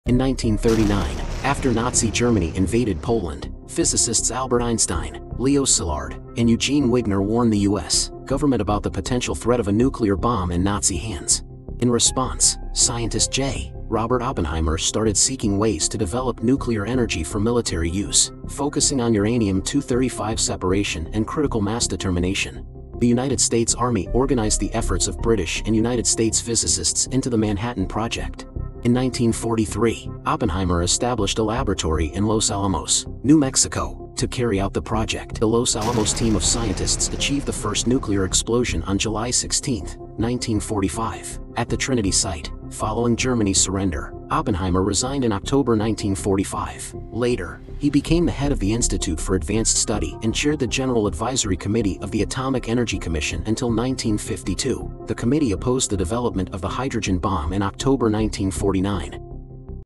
Oppenheimer: The Nuclear Journey
In 1939, after Nazi Germany invaded Poland, physicists Albert Einstein, Leo Szilard, and Eugene Wigner warned the U.S. government about the potential threat of a nuclear bomb in Nazi hands. In response, scientist J. Robert Oppenheimer started seeking ways to develop nuclear energy for military use, focusing on uranium-235 separation and critical mass determination. The U.S. Army organized the efforts of British and U.S. physicists into the Manhattan Project.

In 1943, Oppenheimer established a laboratory in Los Alamos, New Mexico, to carry out the project. However, in 1942, he engaged in discussions with military security agents, leading to implications that some of his associates were Soviet government agents. This resulted in the dismissal of a friend from the University of California faculty. In a 1954 security hearing, Oppenheimer admitted to providing false information during these discussions.

The Los Alamos team of scientists achieved the first nuclear explosion on July 16, 1945, at the Trinity Site. Following Germany's surrender, Oppenheimer resigned in October 1945. Later, he became the head of the Institute for Advanced Study and chaired the General Advisory Committee of the Atomic Energy Commission until 1952. The committee opposed the development of the hydrogen bomb in October 1949.